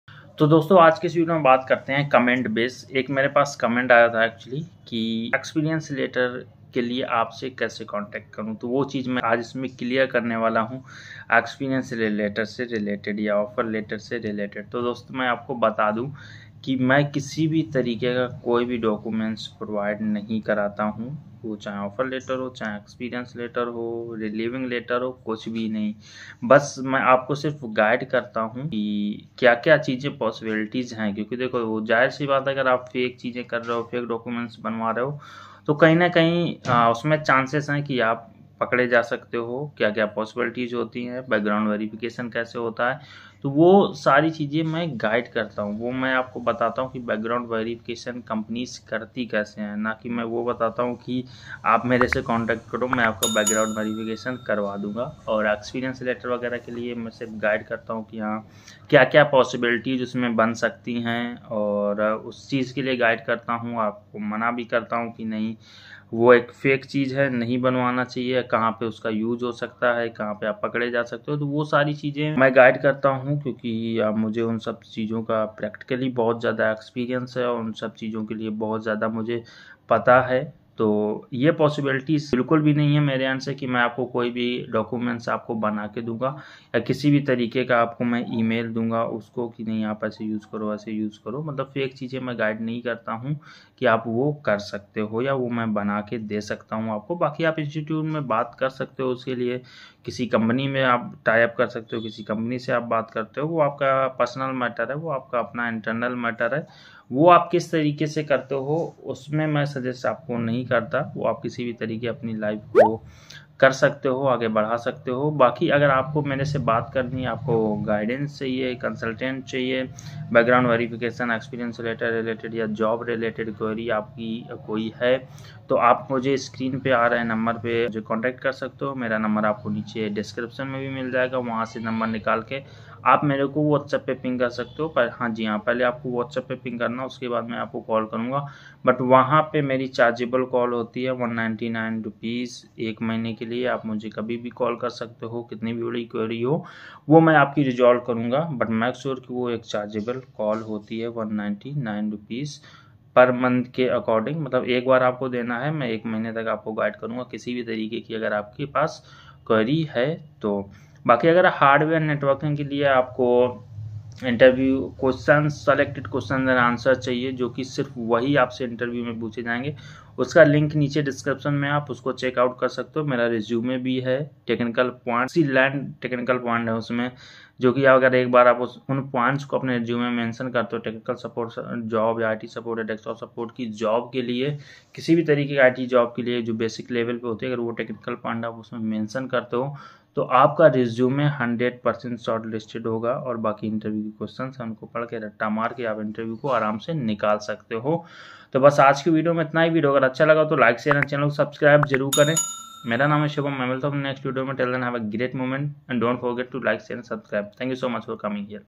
How to Contact me For Experience Letter ?How to make experience/Offer letter | fake experience/offer
Maine is video me btaya h ki aap mere se kaise connect kr sakte ho agr aapko experience letter ya offer letter chahiye hoga to full detail me hindi me discuss kiya h i hope aapko video acha lage thanks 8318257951

Thanks.